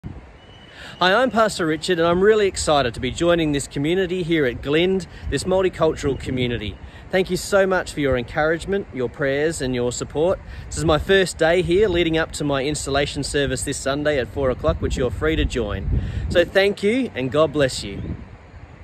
First day at Glynde
I’m excited on my first day at Glynde Lutheran Church.
Richard Fox